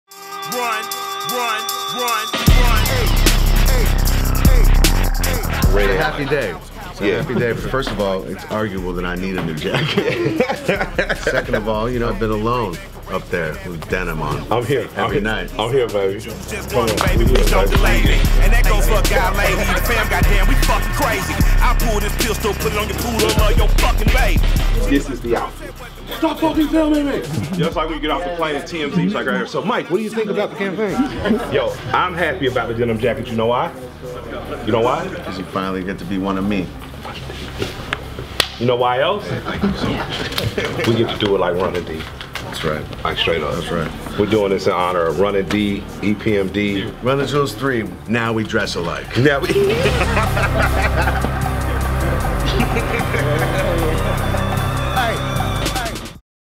Run The Jewels X Volcom - Final Fitting
Another Coachella Festival is in the books and we had a blast.  Volcom is very happy to get in on the fun with brand ambassadors Run The Jewels who preformed on the main stage each Saturday during Coachella.

Run The Jewels approached Volcom with the idea to create custom denim jackets for El-P and Killer Mike as an homage to some of the great rap duos of eras past.  Although the heat of Coachella prohibited Run The Jewels from performing more than 3 or 4 songs in matching denim, they looked great for those moments!

We also collaborated with Run The Jewels and our pals over at The Mountain to create a limited-edition, festival-ready tee shirt of epic proportions.  We have a very small quantity in our webstore for those looking to cop this unique design, but who were unable to attend Coachella.